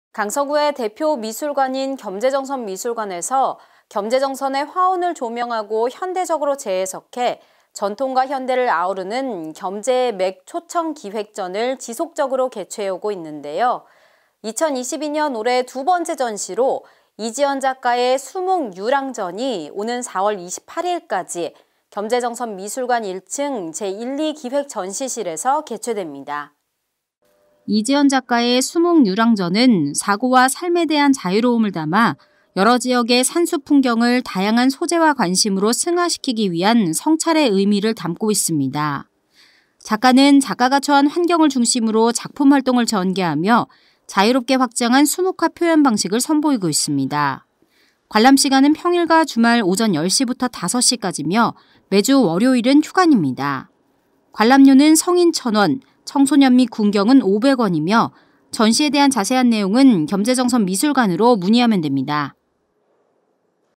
겸재의 맥 초청기획
강서구의 대표 미술관인 겸재정선미술관에서
겸재 정선의 화혼을 조명하고 현대적으로 재해석해
전통과 현대를 아우를 수 있는 ‘겸재의 맥 초청기획’展을
지속적으로 개최해 오고 있는데요.

2022년 올해 두 번째 전시로
이지연 작가의 《수묵유랑_水墨流浪》展이
오는 4월 28일까지 겸재정선미술관 1층,
제1,2기획전시실에서 개최됩니다.

이지연 작가의 《수묵유랑_水墨流浪》展은
사고와 삶에 대한 자유로움을 담아
여러 지역의 산수풍경을 다양한 소재와 관심으로
승화시키기 위한 성찰의 의미를 담고 있습니다.

작가는 작가가 처한 환경을 중심으로 작품 활동을 전개하며
자유롭게 확장한 수묵화 표현 방식을 선보이고 있습니다.

관람시간은 평일과 주말 오전 10시부터 5시까지이며,
매주 월요일은 휴관입니다.

관람료는 성인 1,000원, 청소년 및 군경은 500원이며,
전시에 대한 자세한 내용은
겸재정선미술관(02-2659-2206~7)으로 문의하면 됩니다.